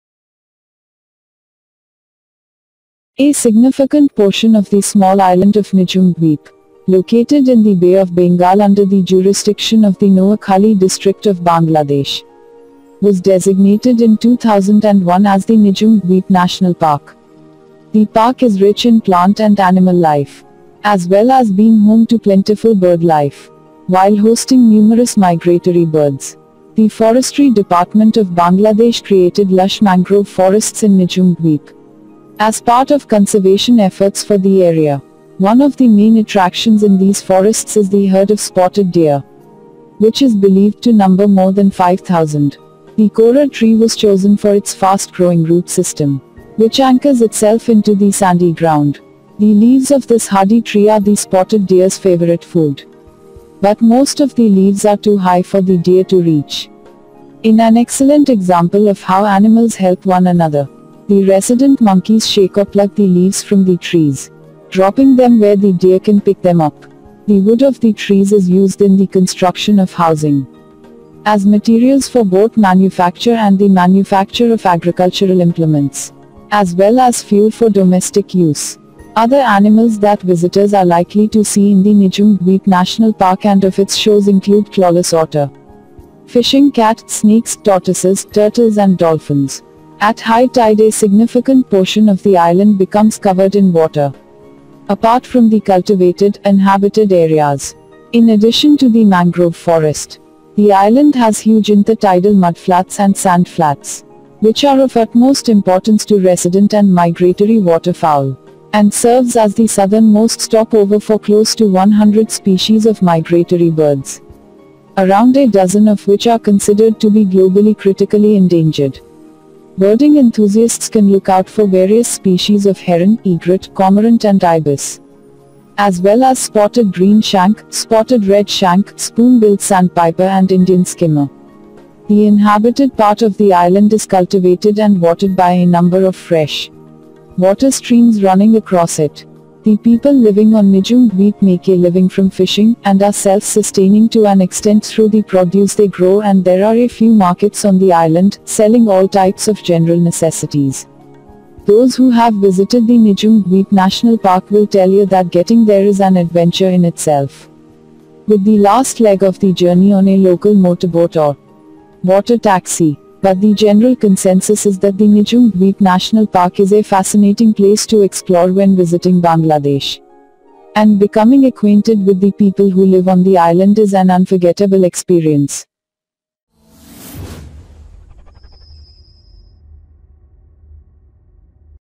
Nijhum Dweep National Park - A Jewel in the Bay of Bengal.
A significant portion of the small island of Nijhum Dweep (also referred to as Nijhum Dwip), located in the Bay of Bengal under the jurisdiction of the Noakhali District of Bangladesh, was designated in 2001 as the Nijhum Dweep National Park. The park is rich in plant and animal life, as well as being home to plentiful bird-life, while hosting numerous migratory birds.

The forestry department of Bangladesh created lush mangrove forests in Nijhum Dweep as part of conservation efforts for the area. One of the main attractions in these forests is the herd of Spotted Deer, which is believed to number more than 5,000. The Keora (or Kerfa) tree was chosen for its fast growing root system, which anchors itself into the sandy ground. The leaves of this hardy tree are the spotted deer’s favorite food, but most of the leaves are too high for the deer to reach. In an excellent example of how animals help one another, the resident monkeys shake or pluck the leaves from the trees, dropping them where the deer can pick them up. The wood of the trees is used in the construction of housing, as materials for boat manufacture and the manufacture of agricultural implements, as well as fuel for domestic use. Other animals that visitors are likely to see in the Nijhum Dweep National Park and off its shores include Clawless Otter, Fishing Cat, Snakes, Tortoises, Turtles and Dolphins.

At high tide a significant potion of the island becomes covered in water, apart from the cultivated, inhabited areas. In addition to the mangrove forest, the island has huge inter-tidal mudflats and sand-flats, which are of utmost importance to resident and migratory water-fowl, and serves as the southern most stop-over for close to 100 species of migratory birds, around a dozen of which are considered to be globally critically endangered. Birding enthusiasts can look out for various species of Heron, Egret, Cormorant and Ibis, as well as Spotted Green Shank, Spotted Red Shank, Spoon-billed Sandpiper and Indian Skimmer.

The inhabited part of the island is cultivated and watered by a number of fresh water streams running across it. The people living on Nijhum Dweep make a living from fishing, and are self-sustaining to an extent through the produce they grow and there are a few markets on the island, selling all types of general necessities.

Those who have visited the Nijhum Dweep National Park will tell you that getting there is an adventure in itself, with the last leg of the journey on a local motorboat or “water-taxi”. But the general consensus is that the Nijhum Dweep National Park is a fascinating place to explore when visiting Bangladesh, and becoming acquainted with the people who live on the island is an unforgettable experience.